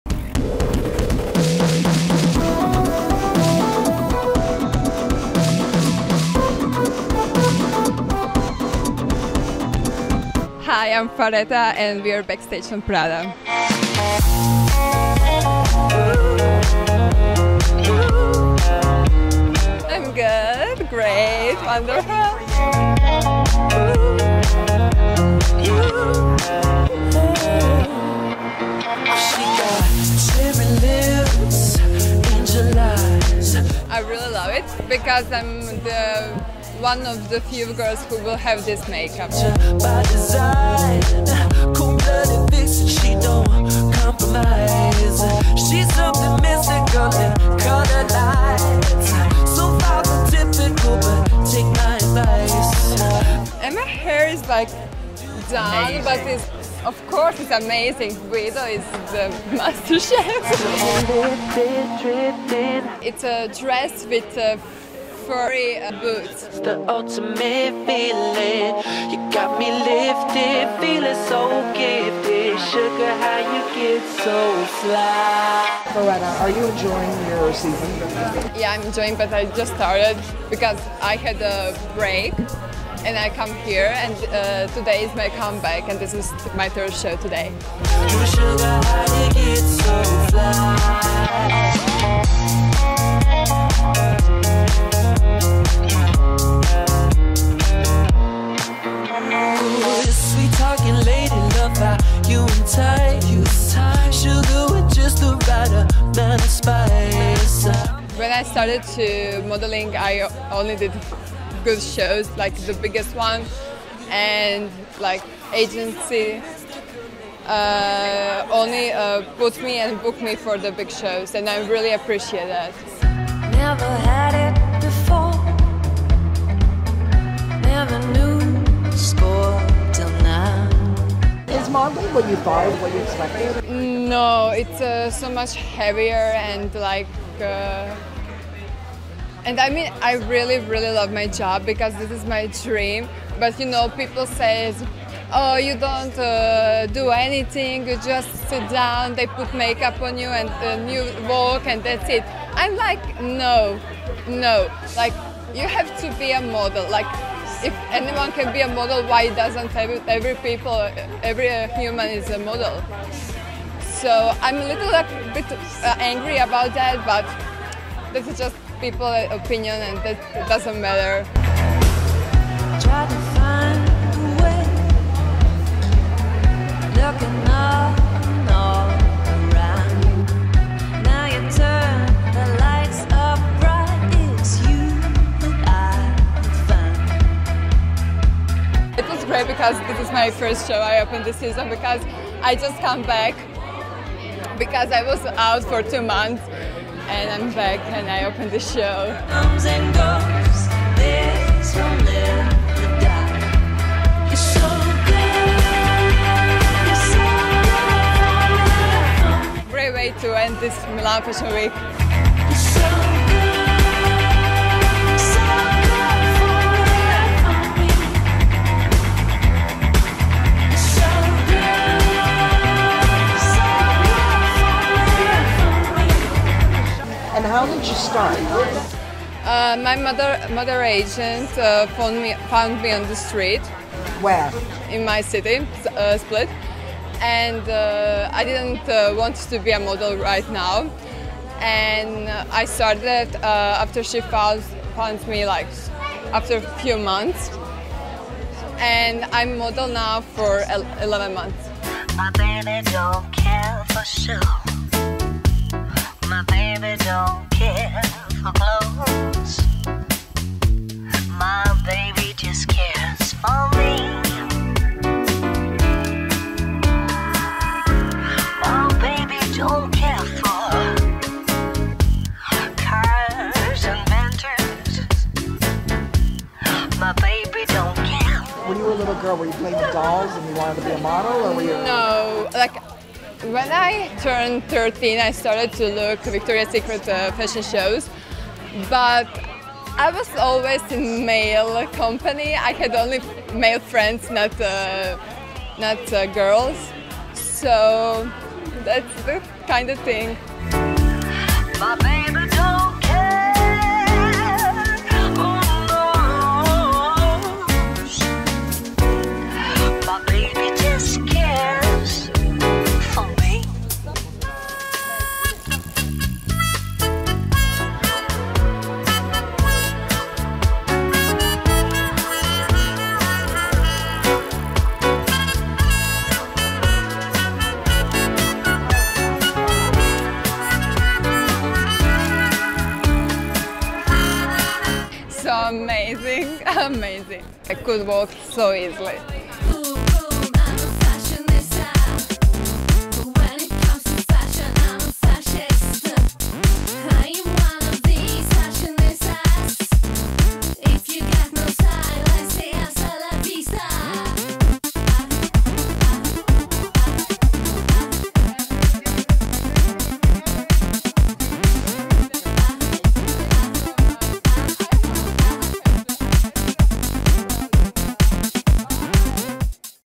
Model Talks Fall/Winter 2017-18 Faretta | FashionTV
WORLD - Faretta shares a day in the life of a model with FashionTV and she brings us fun facts, behind the scenes moments, and incredible fashion. Brought to you exclusively by FashionTV.